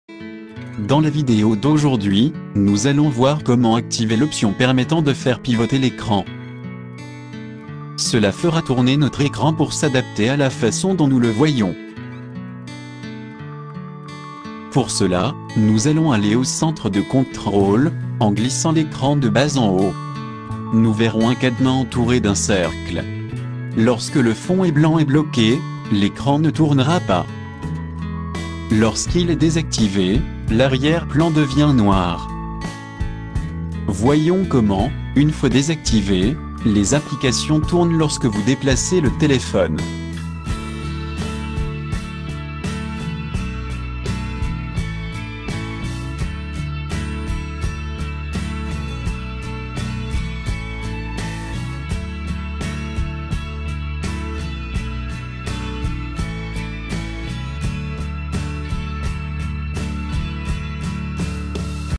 Comment faire pivoter l'écran sur notre iPhone
Didacticiel vidéo dans lequel nous expliquons et expliquons comment activer et désactiver l’option de rotation ou de déplacement de l’écran de notre téléphone. De cette manière, la rotation nous permettra d’adapter les applications à notre écran.
Valable pour iPhone 4, 4S, 5S, 5S, 5C, 6, 6 Plus, 6S, 6S Plus, 7, 7 Plus, SE, 8, 8 Plus, X, XS, XR, XS et pour iPad.